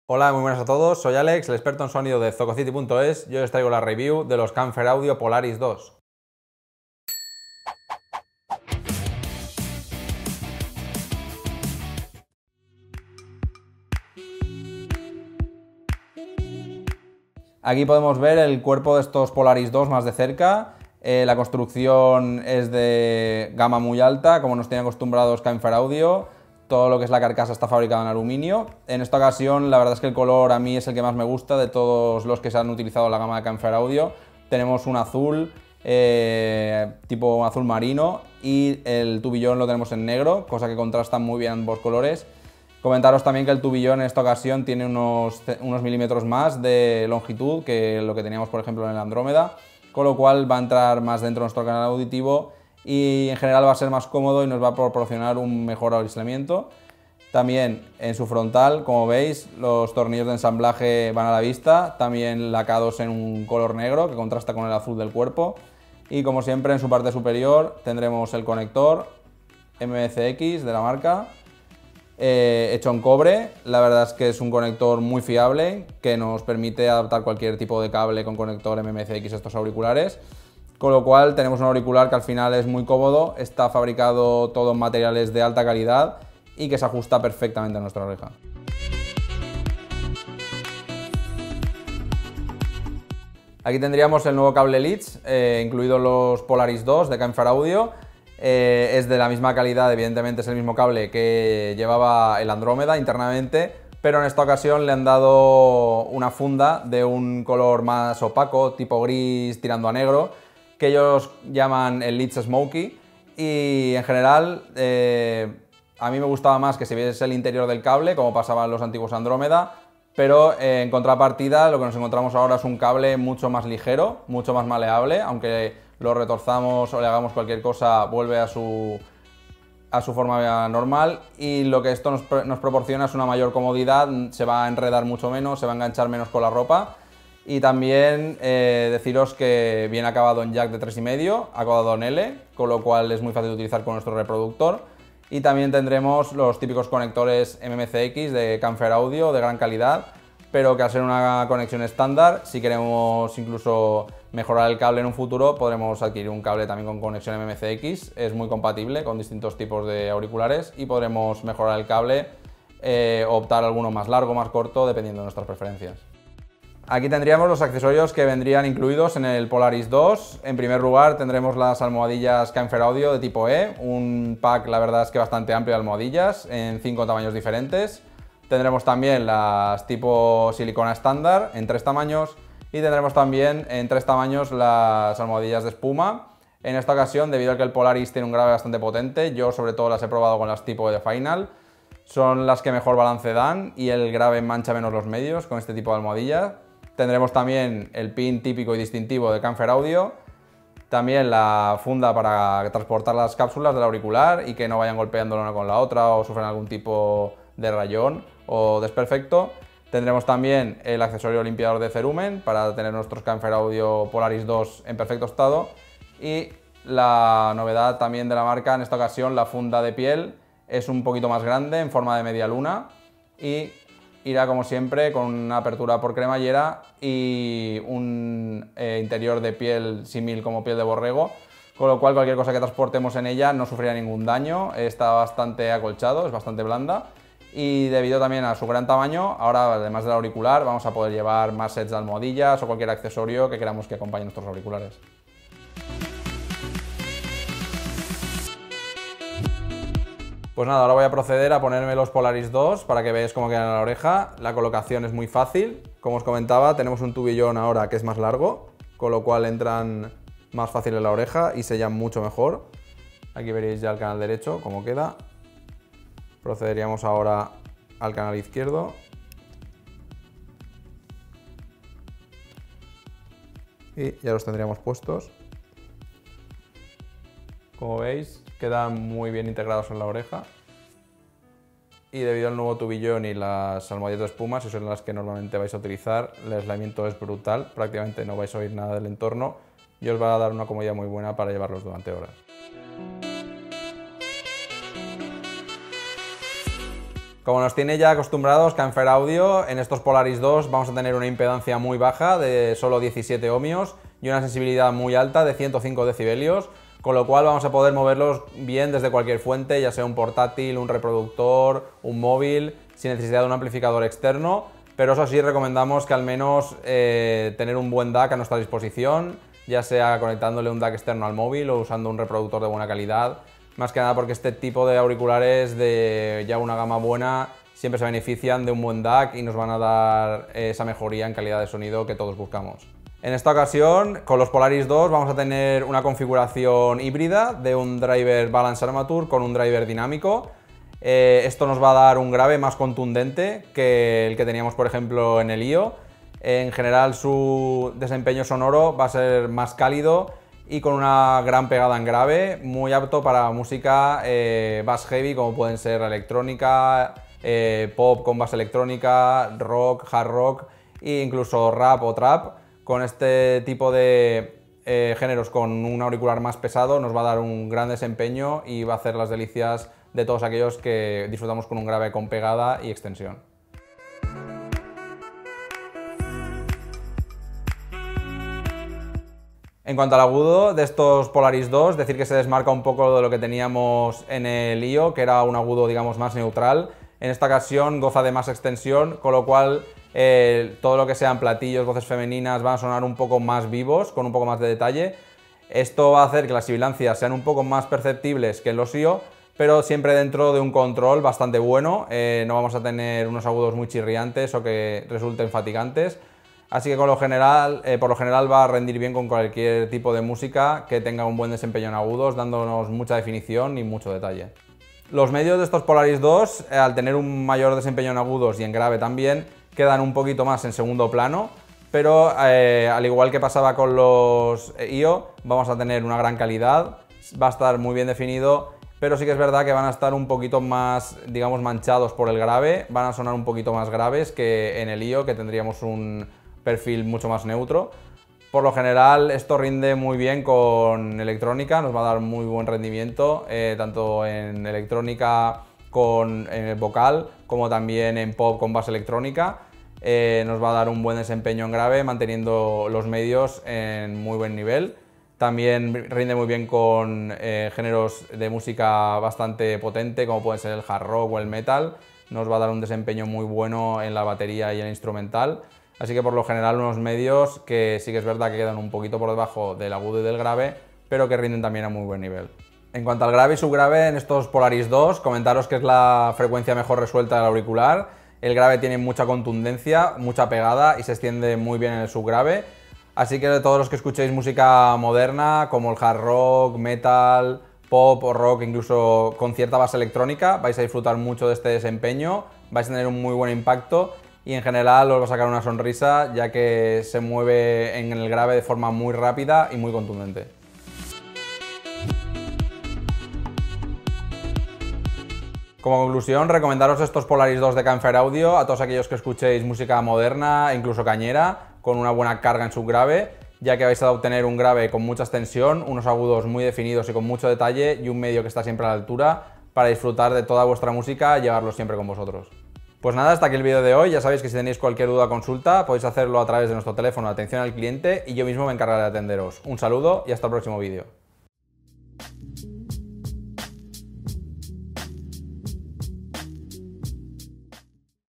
Review Campfire Audio Polaris II, la nueva gama Campfire pisa fuerte
Los Campfire Audio Polaris II capitanean la gama media de la nueva serie de auriculares Campfire, con un diseño muy especial en color azul, y las frecuencias graves como punto fuerte.

► Comprar Campfire Audio Polaris II en España al mejor precio. Distribuidor oficial